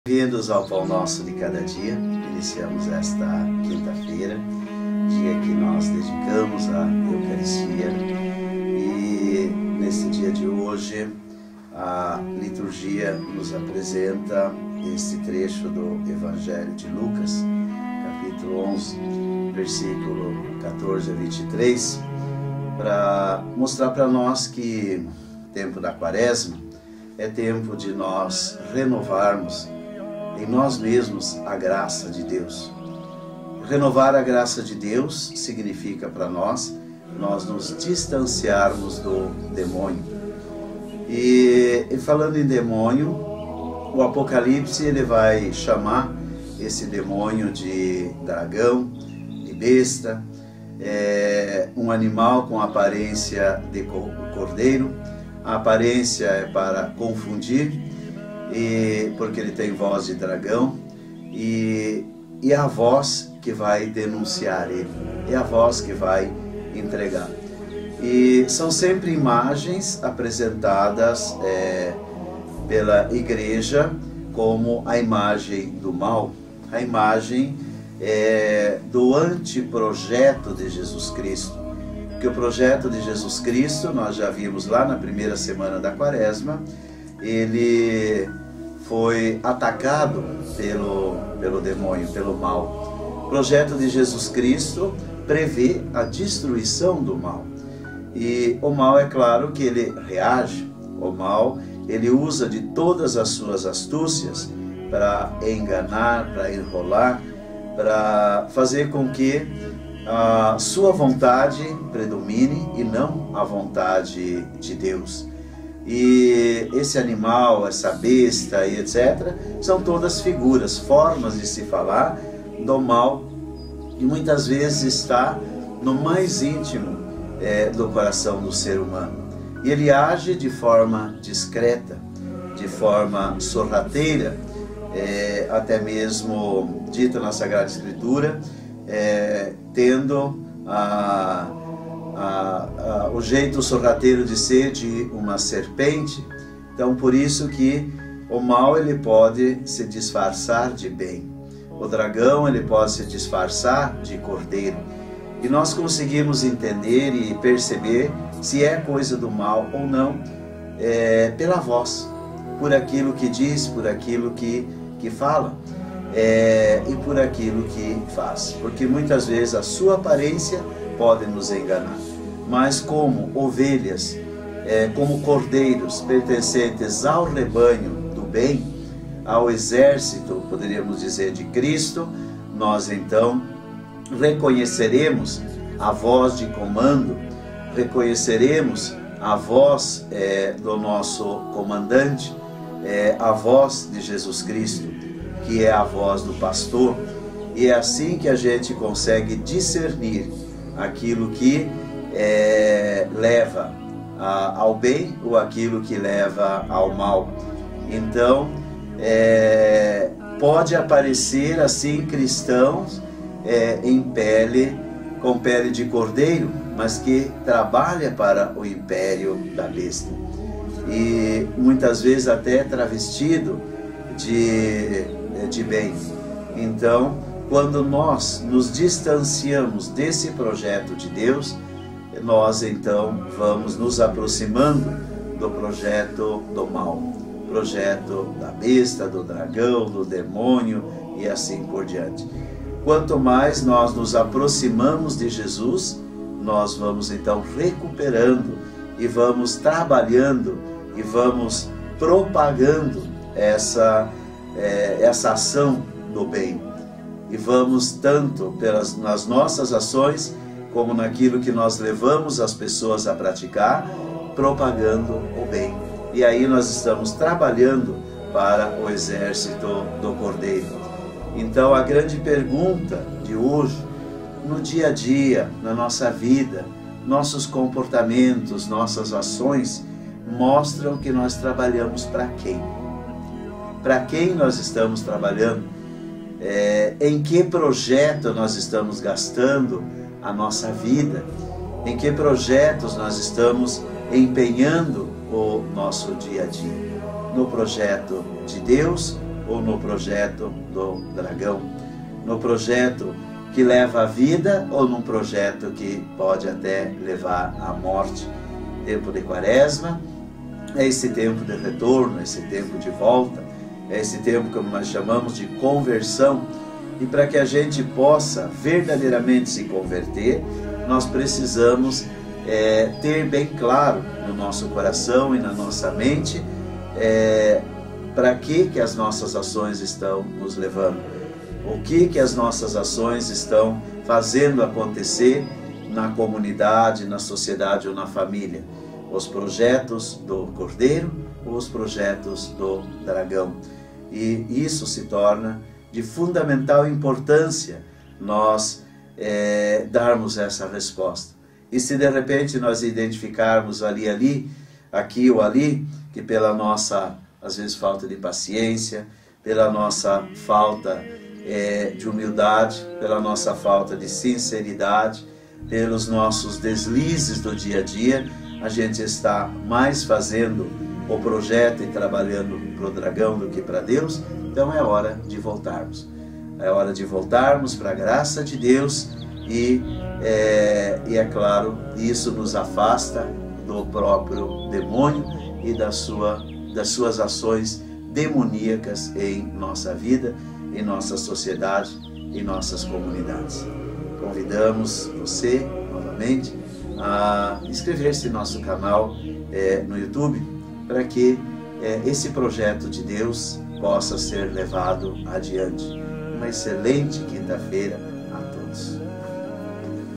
Quaresma é Tempo de Renovarmos em Nós a Graça de Deus.
Evangelho de - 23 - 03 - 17 Qui - Quaresma é tempo de renovarmos em nós a graça de Deus e distanciar-nos do demônio. O Apocalipse chama o demônio de dragão, besta, um animal com a aparência de cordeiro para confundir e voz de dragão que o denuncia. Assim pode haver cristãos com pele de cordeiro mais que trabalham para o império da besta. Trabalhamos para quem?